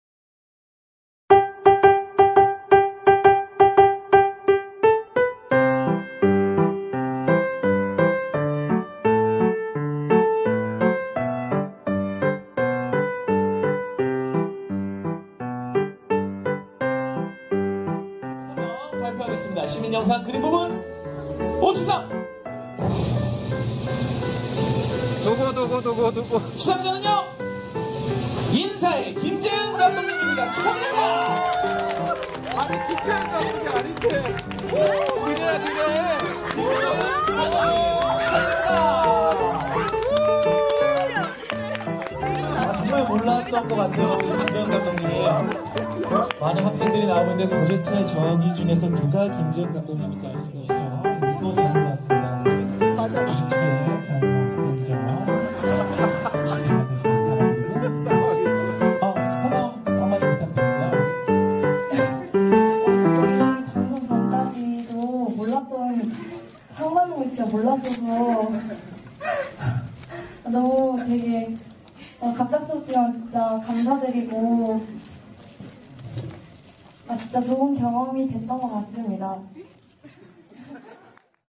꿈즈 3기 우리영상보라고 팀'인사' 한국영상문화제전2016 수상 영상
꿈즈 3기 우리영상보라고팀의 작품 '인사'가 2016 한국영상문화제전에서 우수상을 수상했습니다. 기쁨에 가득찬 우리 청소년 감독들의 수상 현장을 담아왔습니다^^